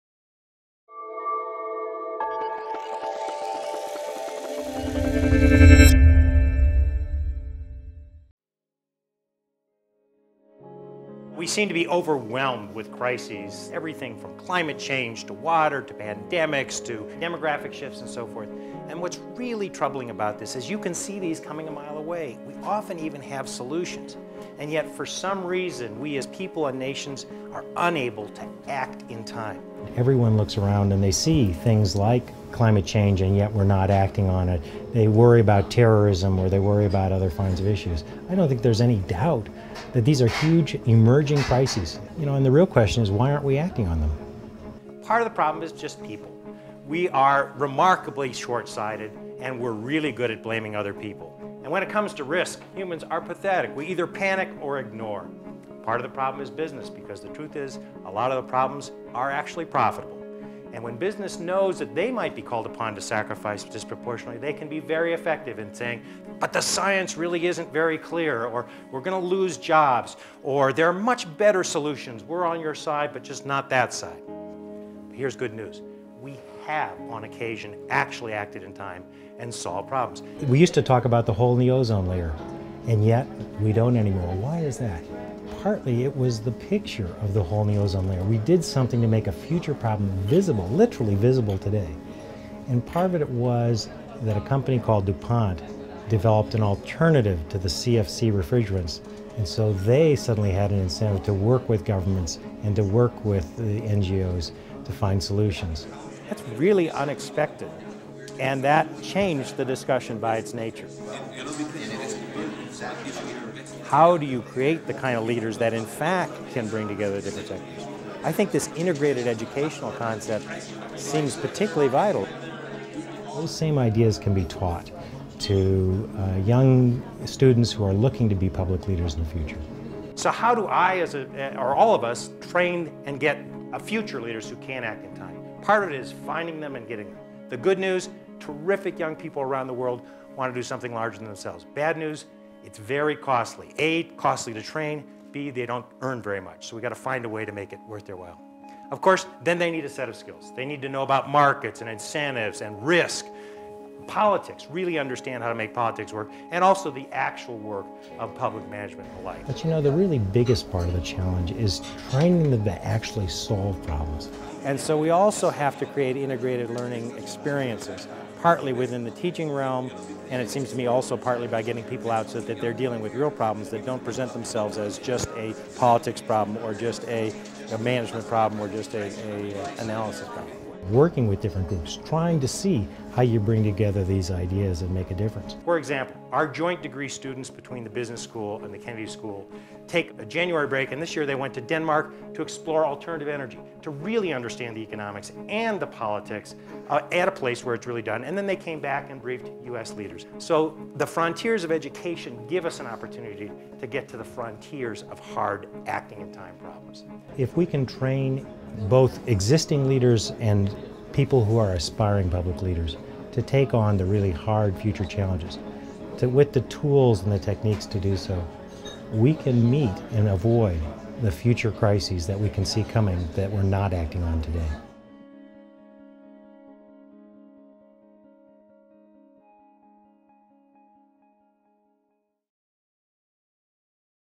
Ideas @Davos | David J Ellwood | Redesigning Risk Response
'When it comes to risk, [...] we either panic or ignore'
When managing risks, leaders need to be able to foster and mitigate some our instictive aversion to acknowledging danger.